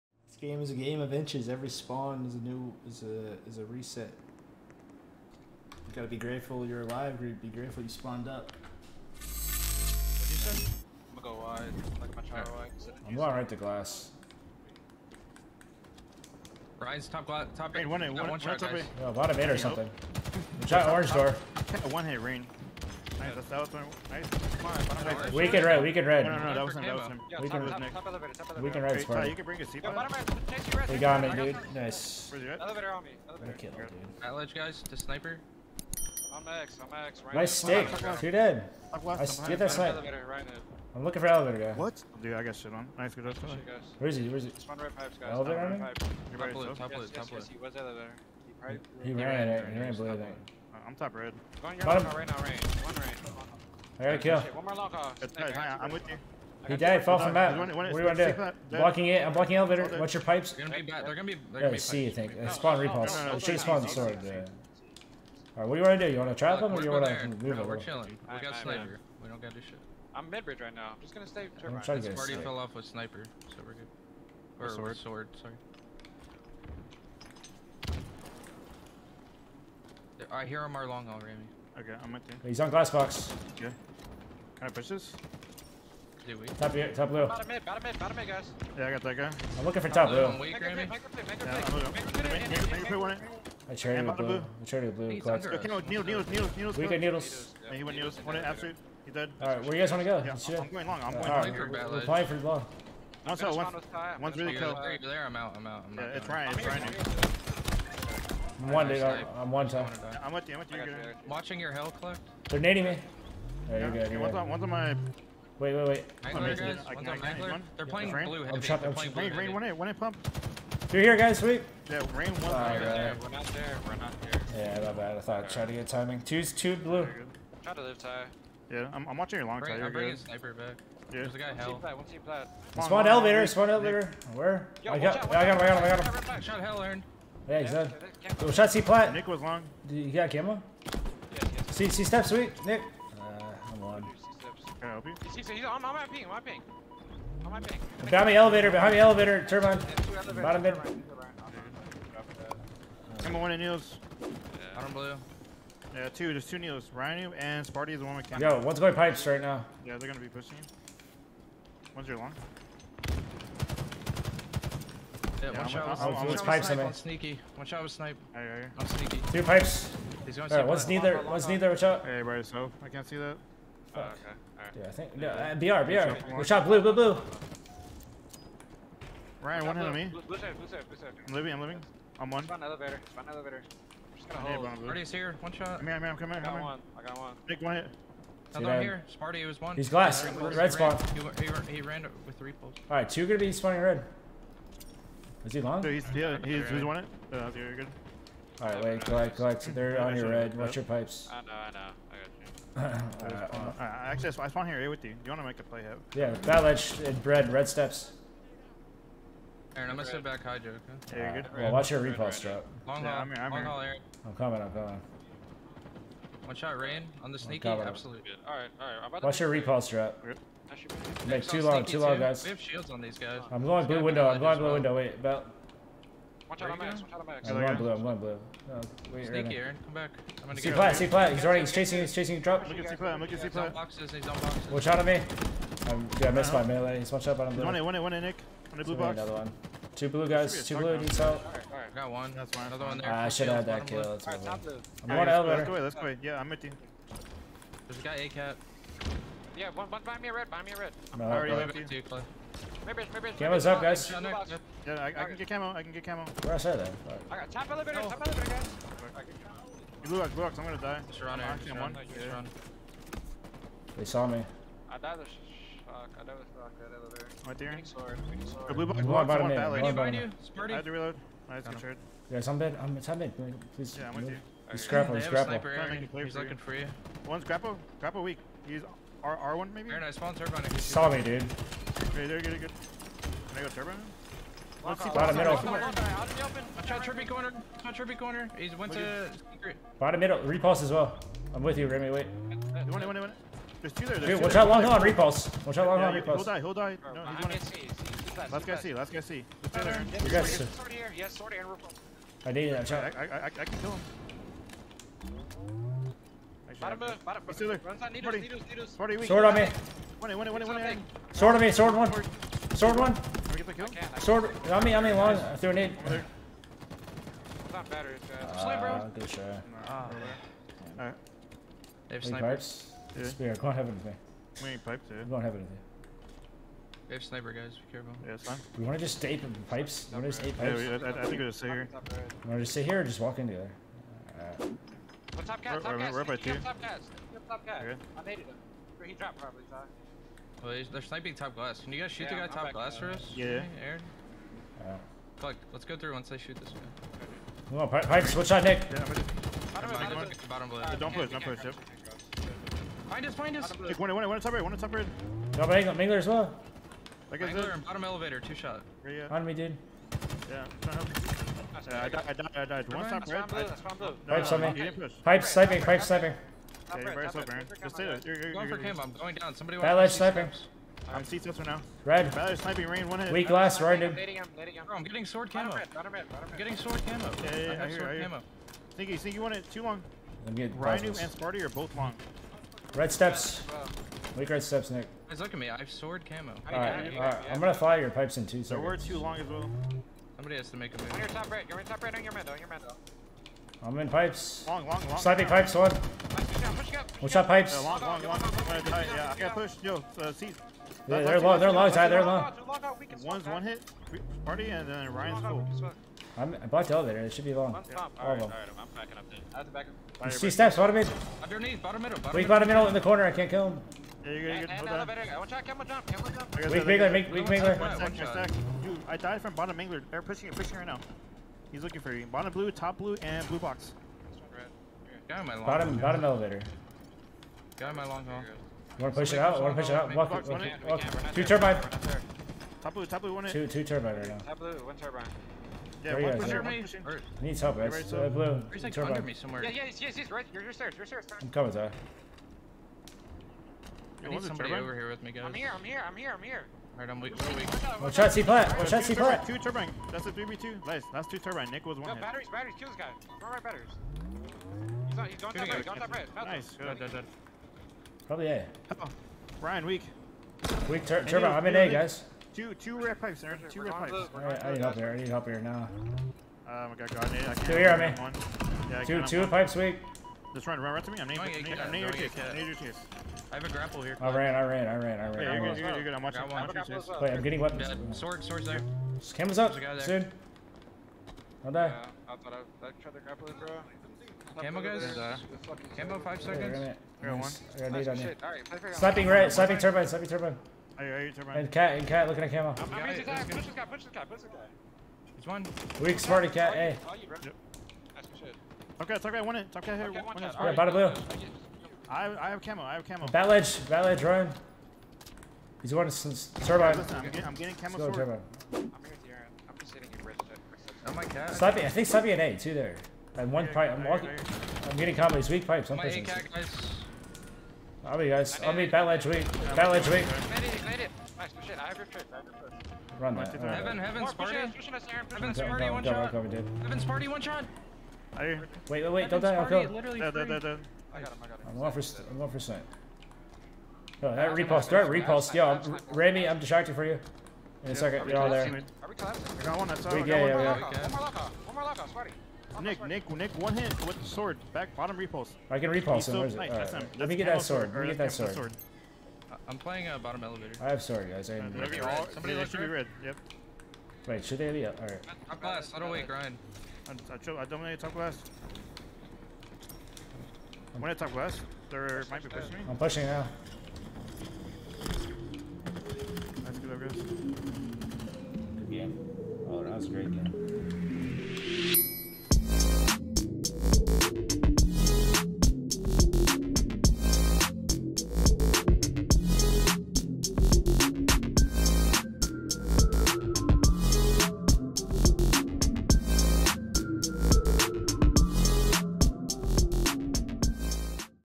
New SSG Roster Making All the Right Moves in Scrims
How do you think we are looking so far since picking up rammy and kuhlect?